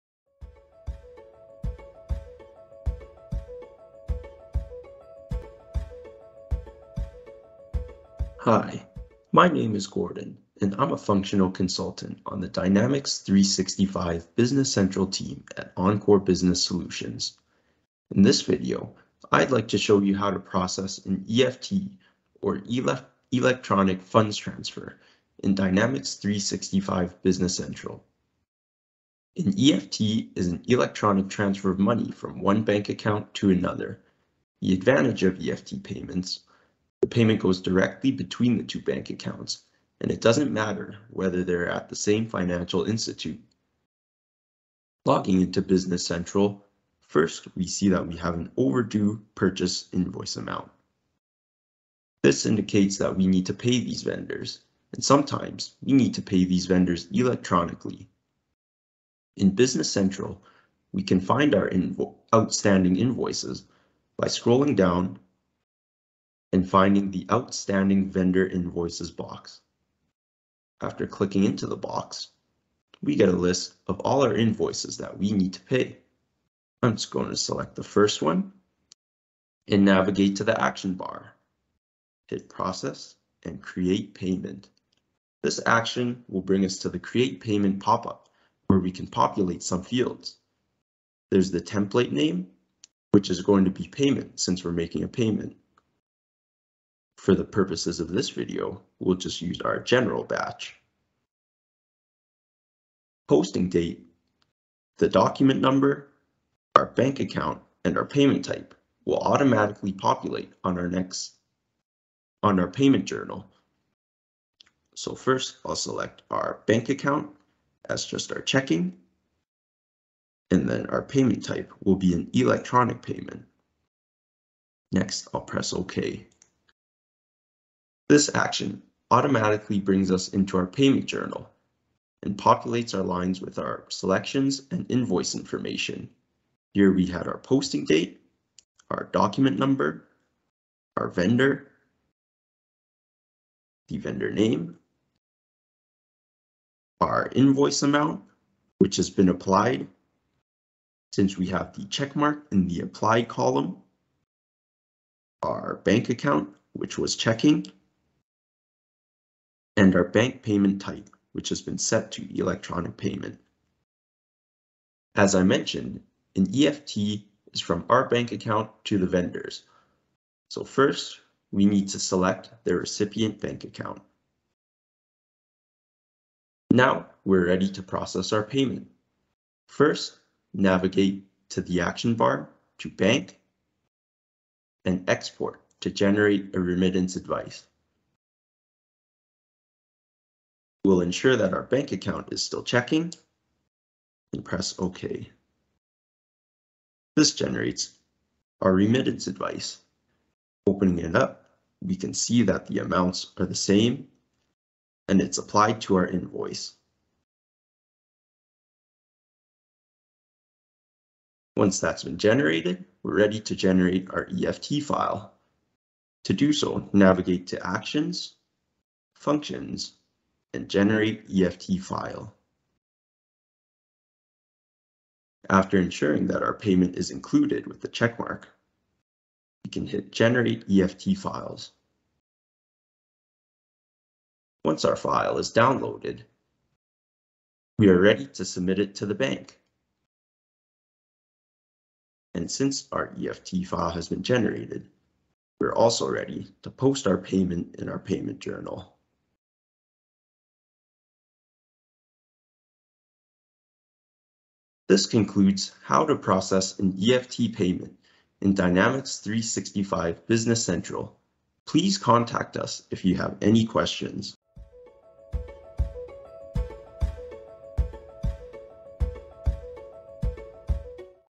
How to Process an EFT Payment in Dynamics 365 Business Central
Learn how to process an EFT payment in Dynamics 365 Business Central. An EFT is an electronic transfer of money from one bank account to another. The payment goes directly between the bank accounts, regardless of if they're at the same financial institute.